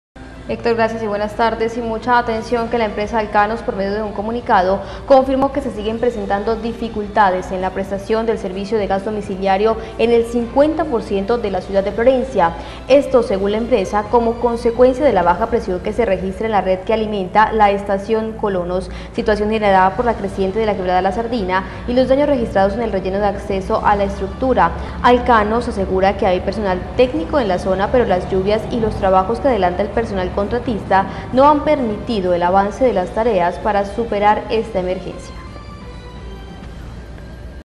Alcanos dio a conocer que aún hay problemas en la prestación de servicio de gas
Alcanos dio a conocer que aún hay problemas en la prestación de servicio de gas en Florencia
NOTICIAS TV5